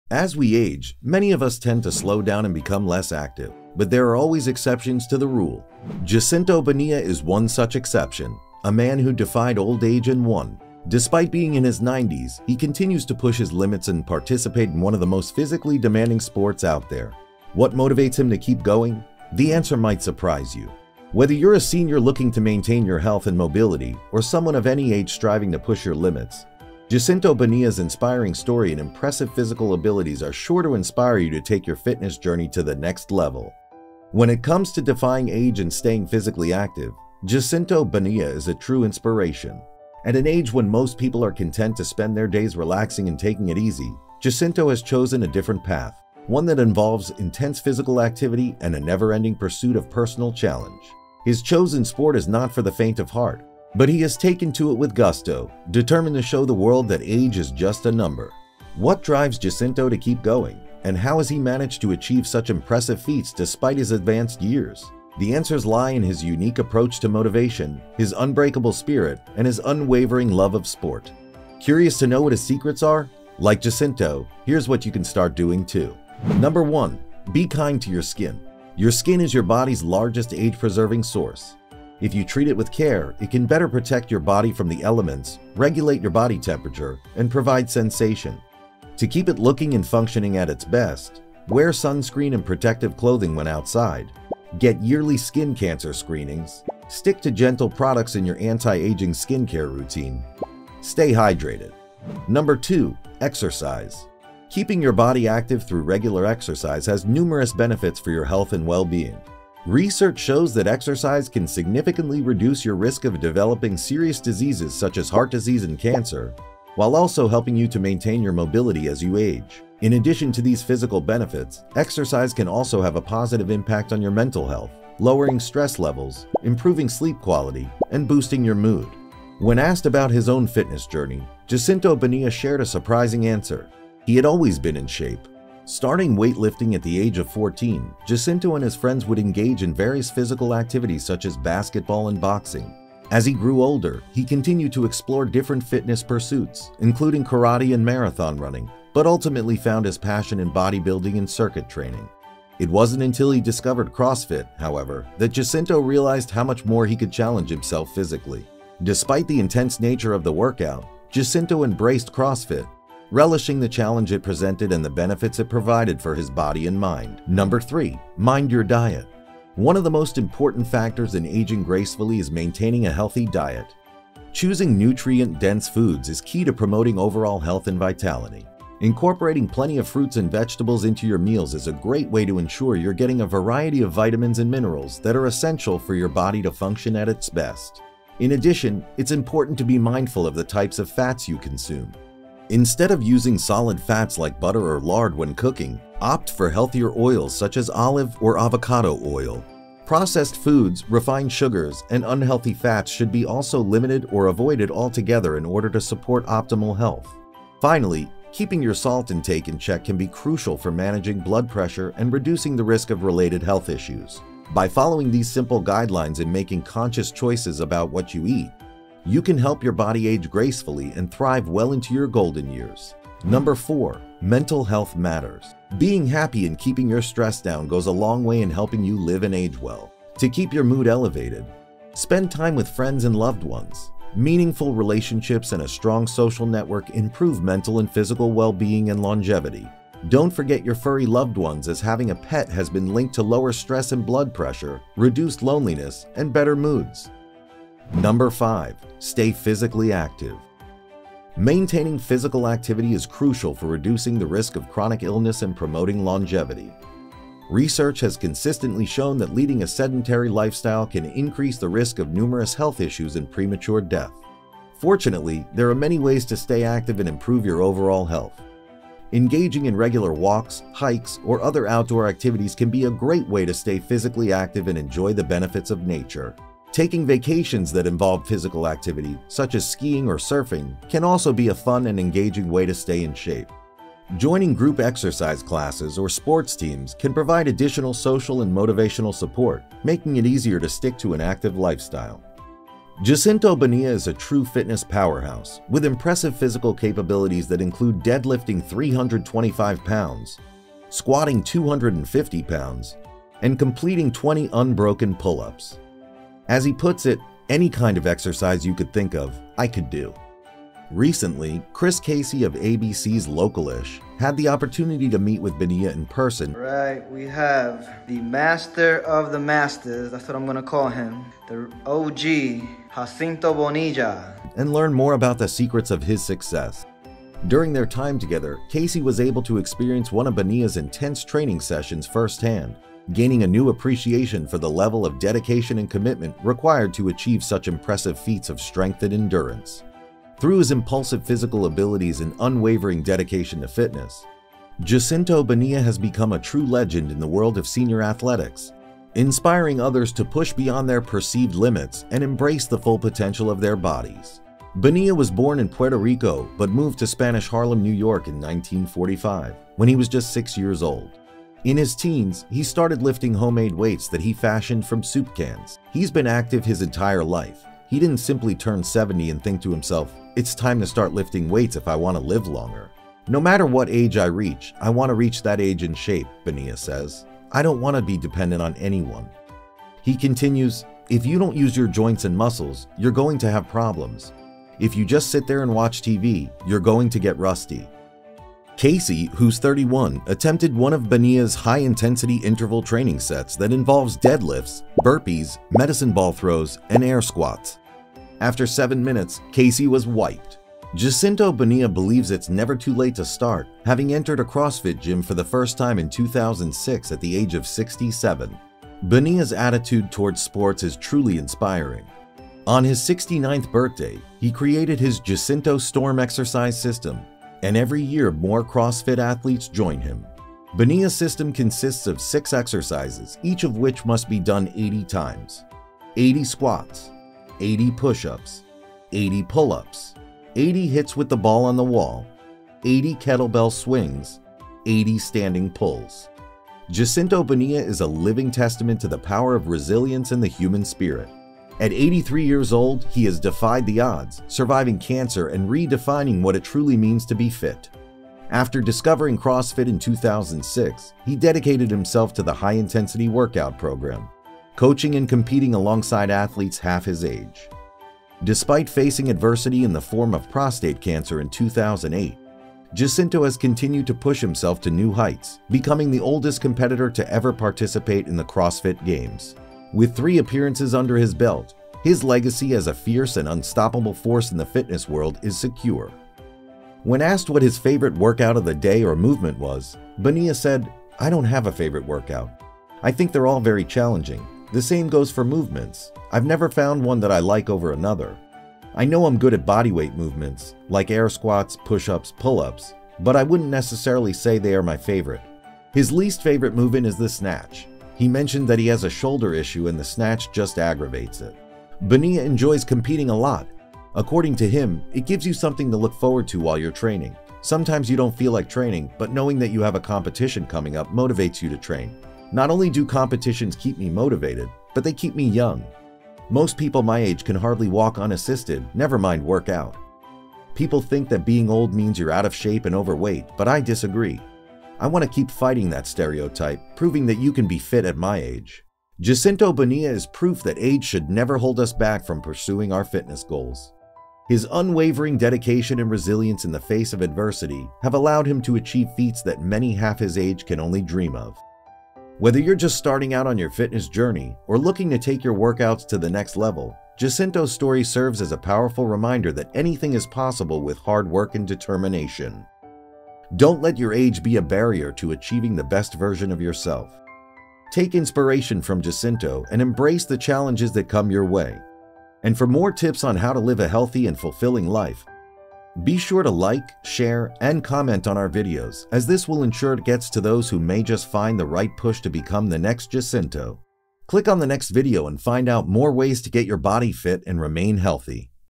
Jacinto Bonilla: The 86-Year-Old Who Proves Age is Just a Number in CrossFit!
Looking for information about Jacinto Bonilla, the legendary fitness trainer and inventor of the kettlebell swing? Look no further than this video!

In this video, we explore the life and accomplishments of Jacinto Bonilla, a pioneer in the world of fitness and kettlebell training. From his early days as a competitive bodybuilder to his groundbreaking work with kettlebells, Bonilla has dedicated his life to helping others achieve their fitness goals.

With expert insights and never-before-seen footage, this video is the ultimate guide to Jacinto Bonilla and his contributions to the world of fitness. Whether you're a seasoned athlete or just starting out on your fitness journey, you won't want to miss this informative and inspiring video.

So what are you waiting for? Hit play and discover the incredible story of Jacinto Bonilla, a true fitness icon and inspiration to millions around the world.